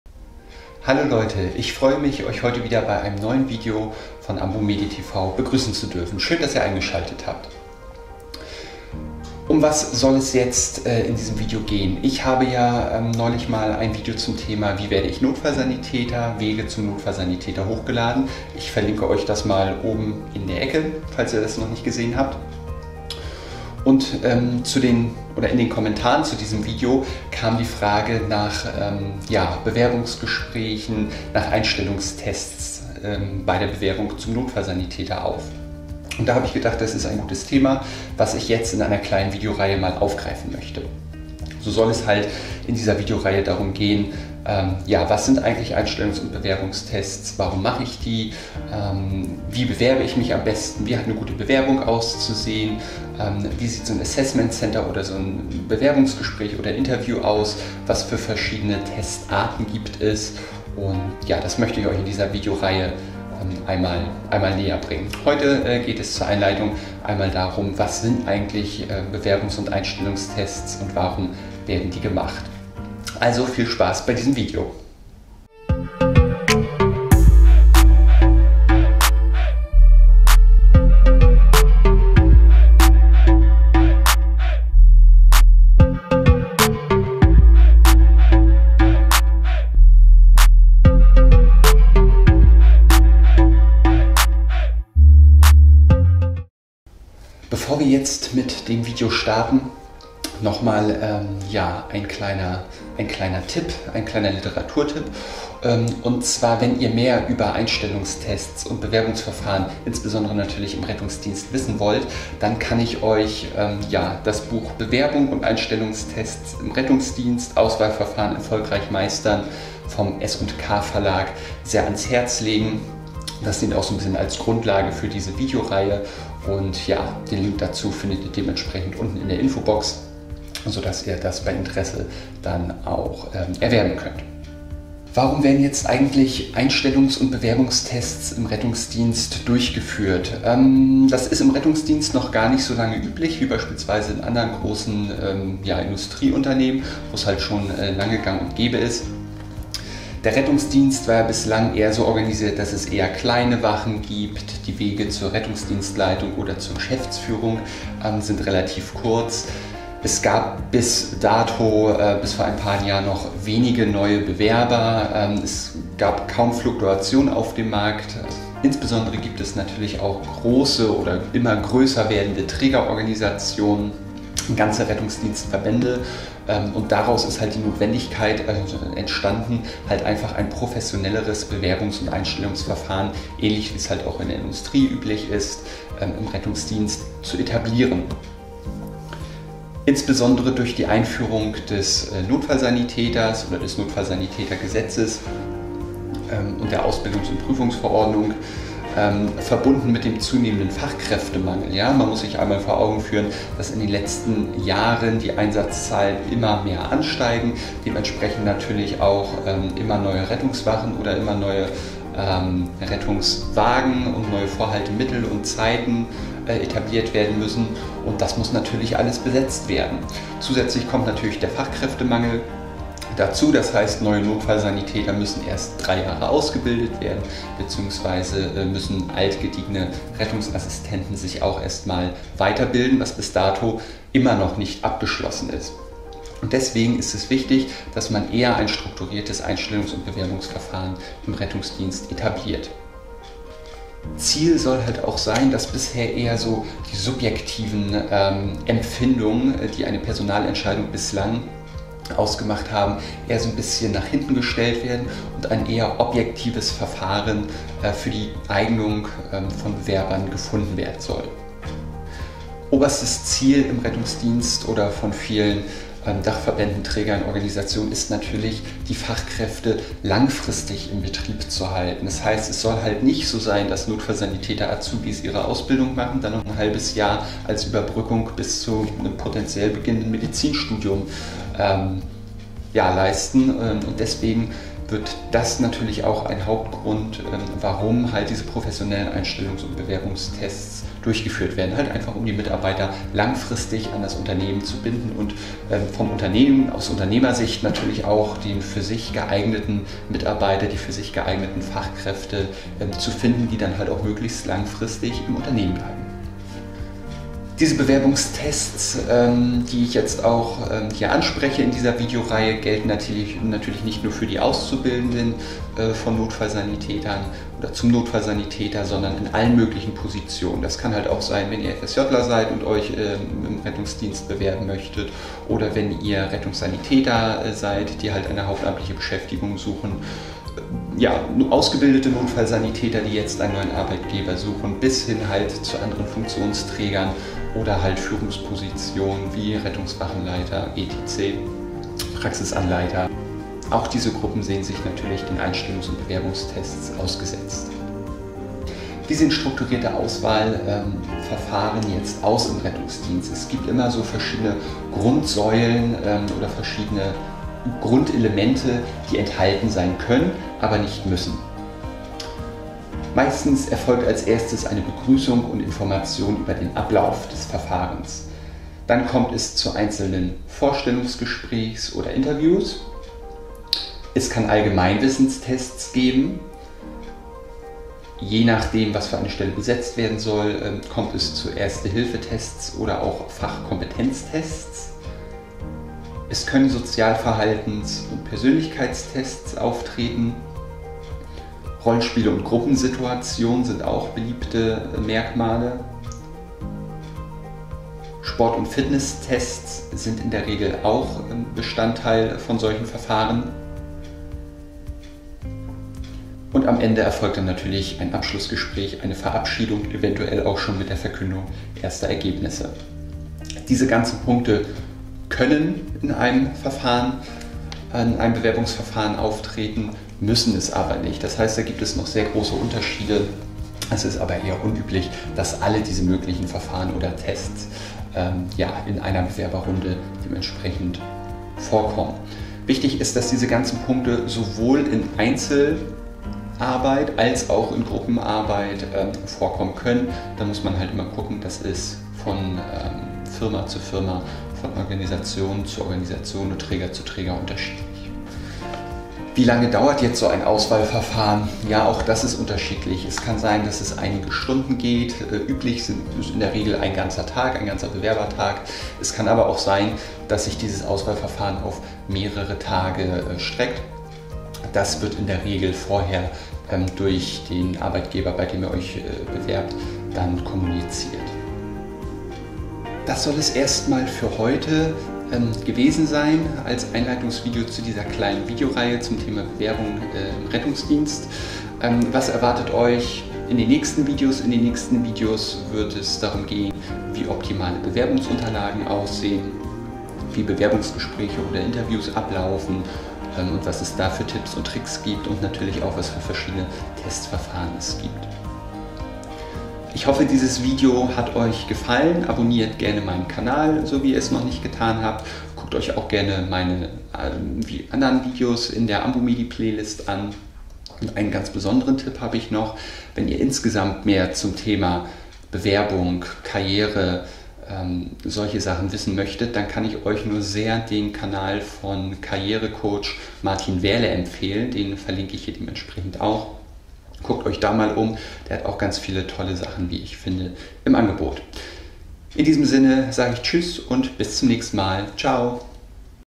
Bewerbungs- und Auswahlverfahren im Rettungsdienst - Grundlagen (Teil 1/6)
Mit diesem Video starte ich die Serie "Bewerbungs- und Auwahlverfahren im Rettungsdienst". Im ersten Teil geht es um die Grundlagen der Einstellungstests - warum gibt es sie, was beinhalten sie, wie laufen sie ab.